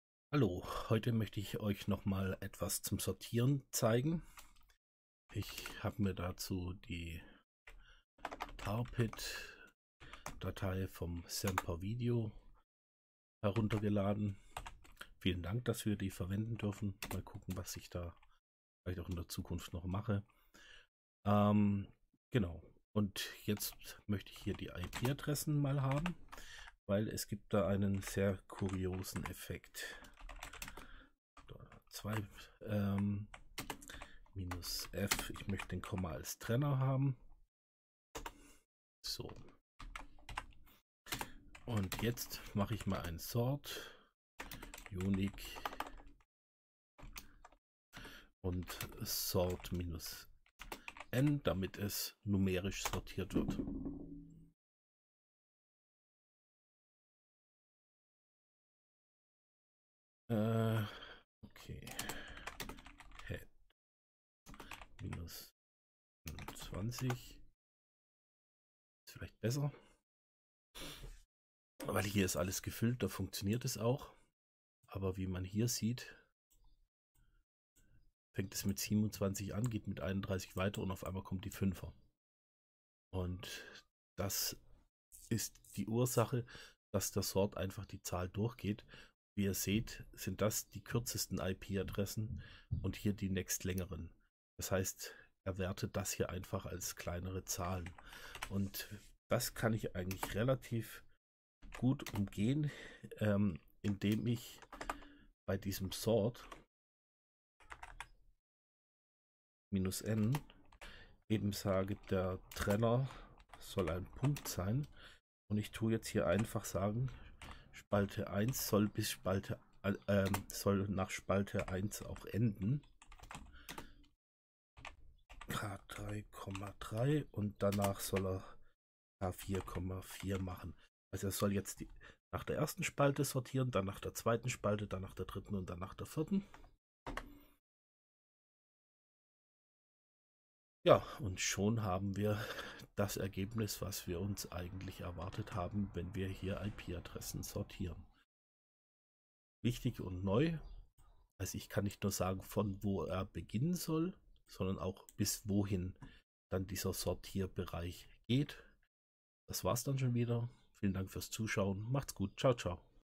#28 IP Adressen sortieren - Probleme mit sort - Datenaufbereitung in der Bash
Ein sort -nr sortiert leider IP-Adressen nicht richtig. In diesem Video zeige ich euch, wie man das Problem lösen kann.